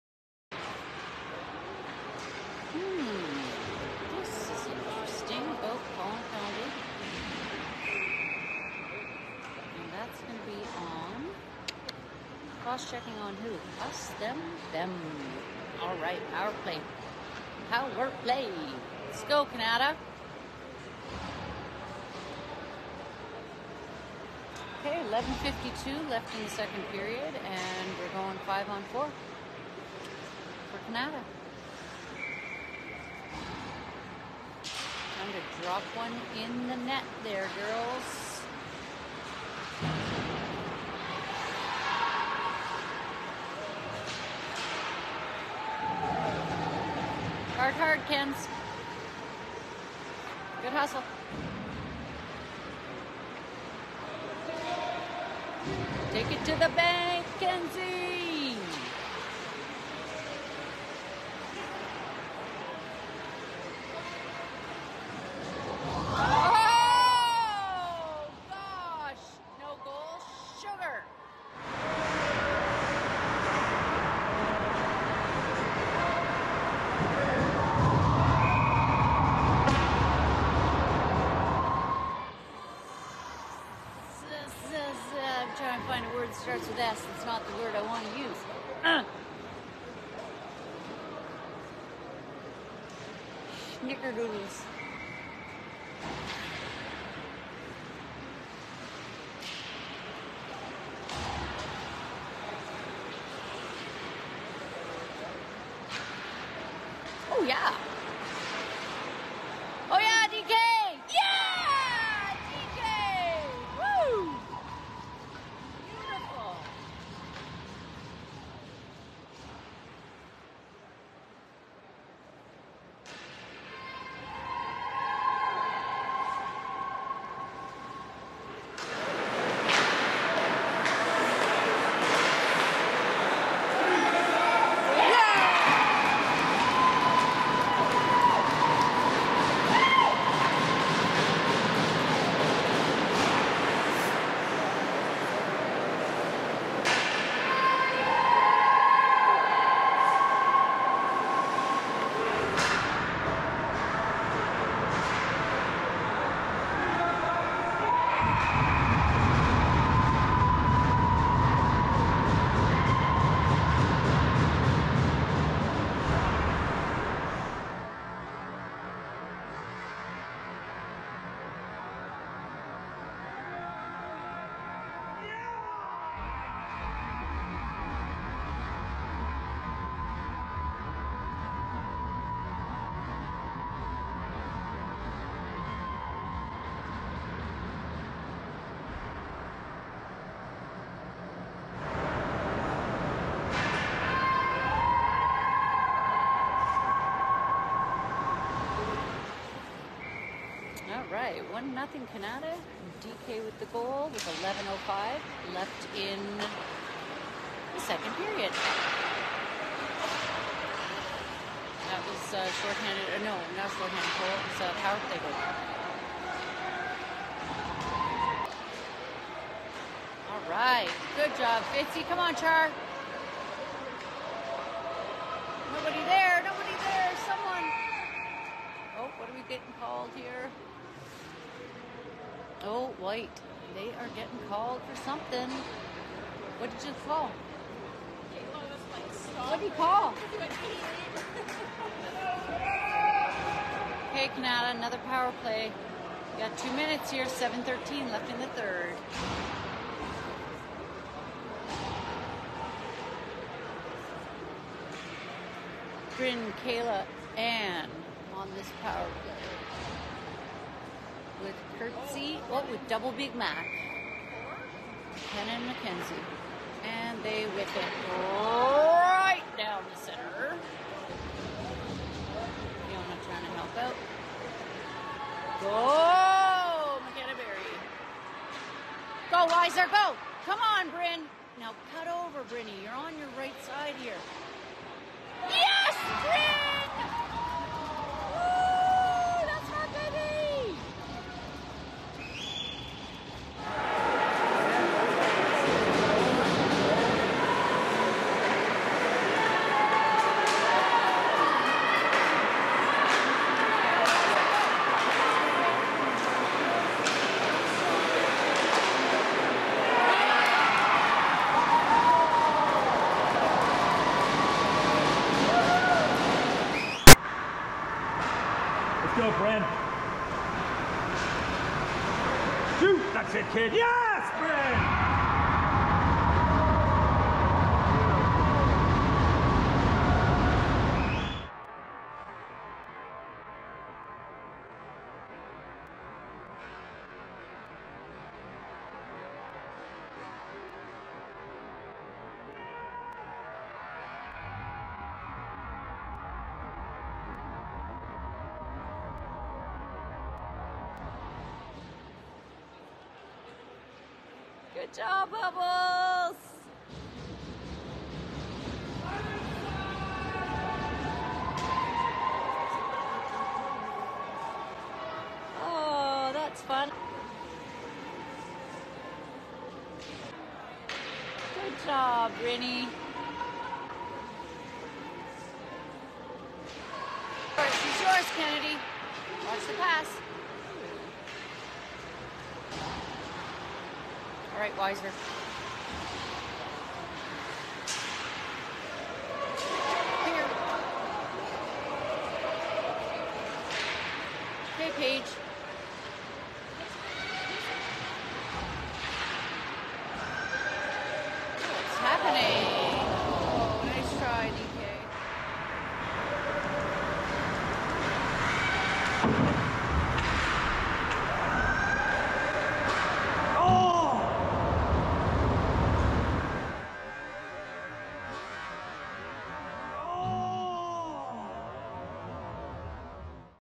Power Play Goal Montage - 2024-01-10 U18BB vs Nepean T2-2
0:00:00 Powerplay Montage
0:00:58 Kirtz almost scores
0:01:37 Goal: DK Assists: Shiy, Kirtz
0:04:30 Goal: Brynn Assists: Anne, Makenna
0:06:16 Bonus: The one that got away